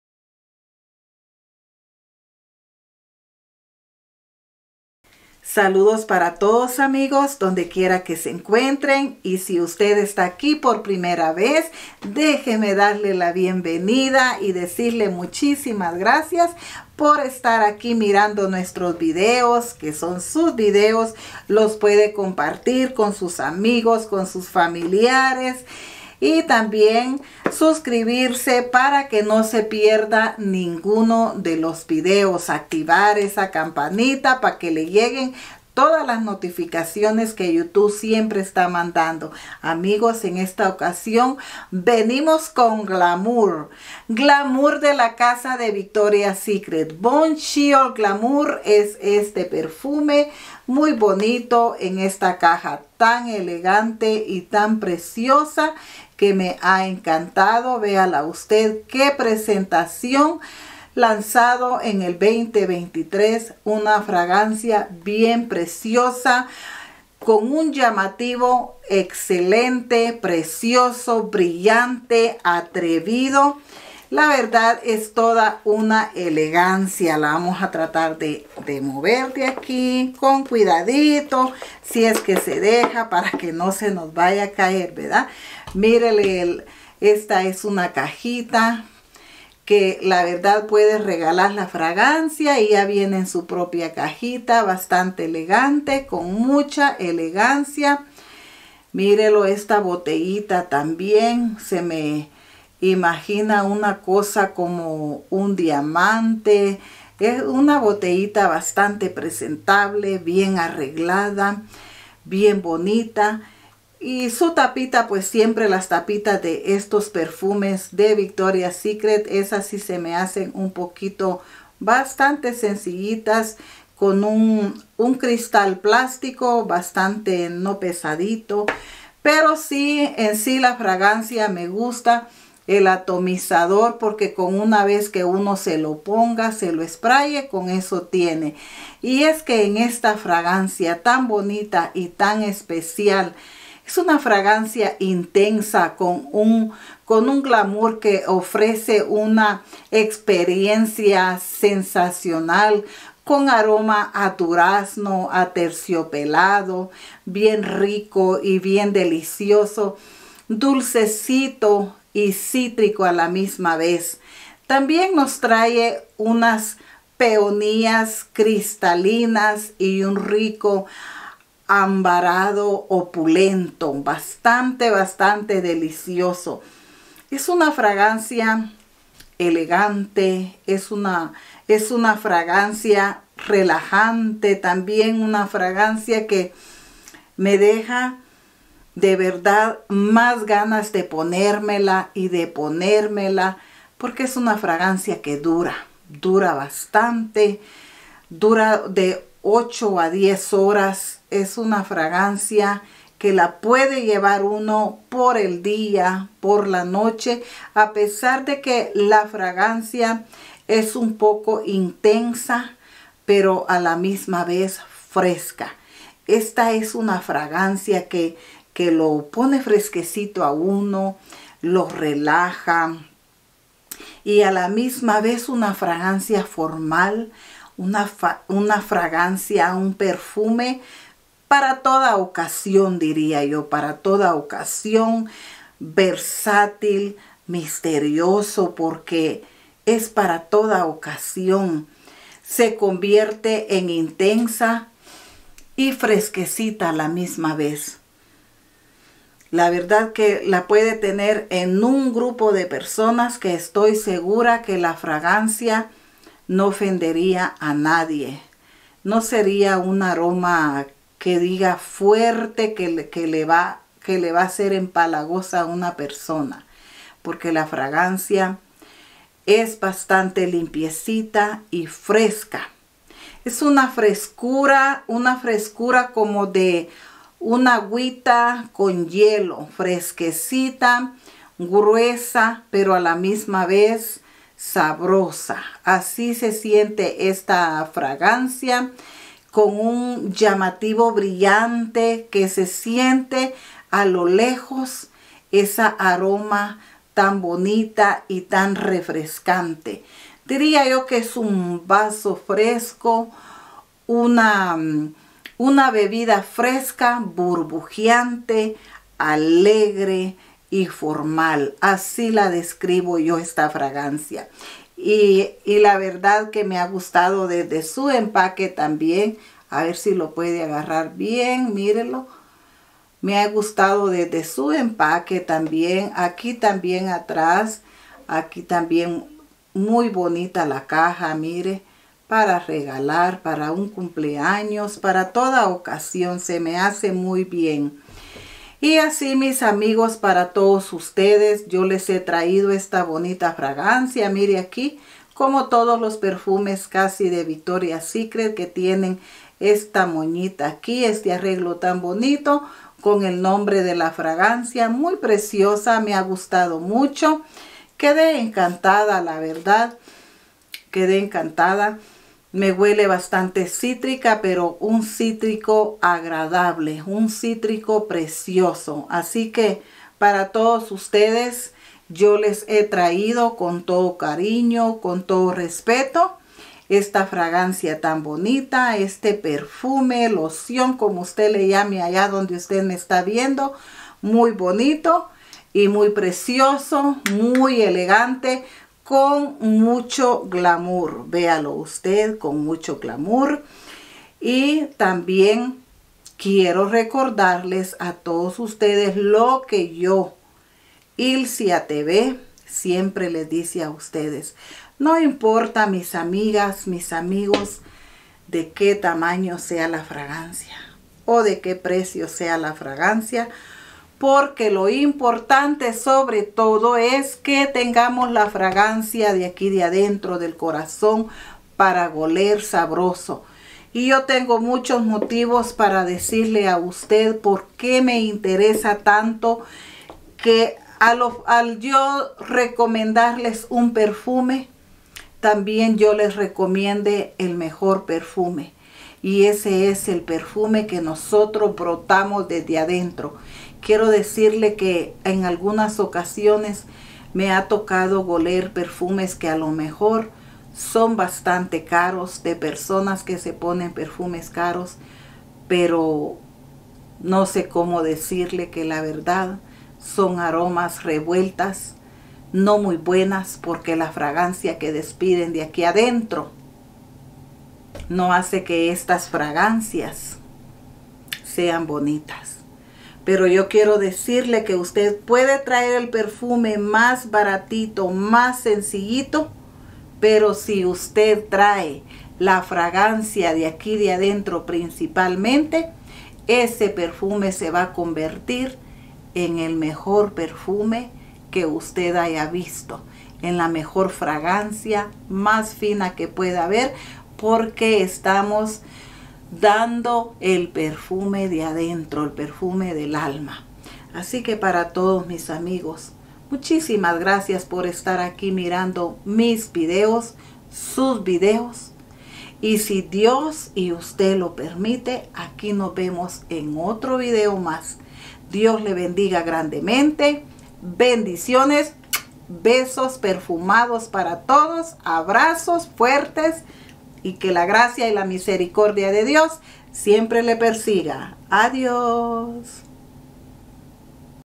VICTORIA'S SECRET BOMBSHELL GLAMOUR
Get ready to unleash your inner bombshell with Victoria's Secret Bombshell Glamour! This fragrance combines exotic and fresh notes to create a unique and alluring scent. Feel confident, elegant and empowered with just one spritz. Perfect for the femme fatale in all of us. Try it now and experience the ultimate glamour!